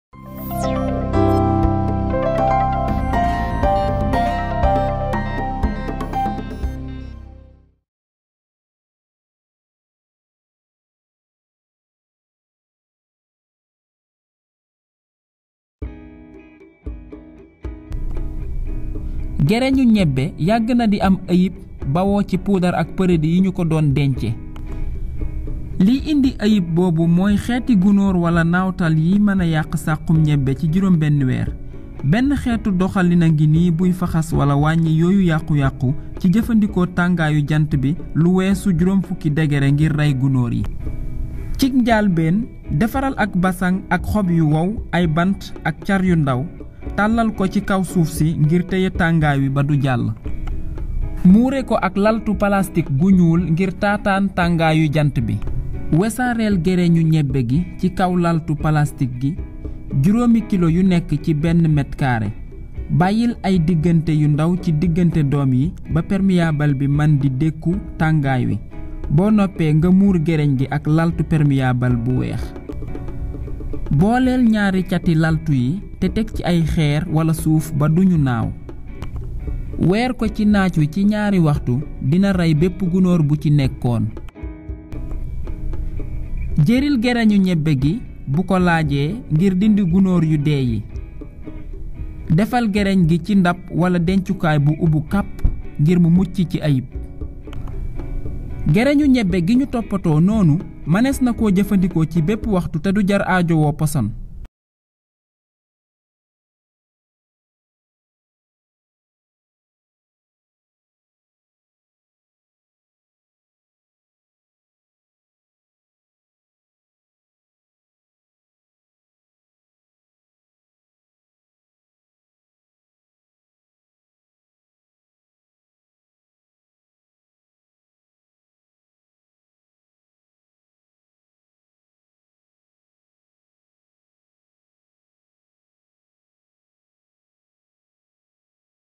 Aaar ñebbe ak tangoru jant bi (ci Wolof)
La conservation du niébè avec le séchage solaire en wolof  (accent du Sénégal)

Solar Treating of Cowpea Seeds in Wolof (accent from Senegal)

 Cowpea bruchids (Callosobruchus maculatus) cause damage to cowpea (Vigna unguiculata) seeds in storage,  resulting in post-harvest losses. In the animated video,  which can be used on smart phones,  we describe a solarization technique to kill cowpea bruchids before the seeds are stored.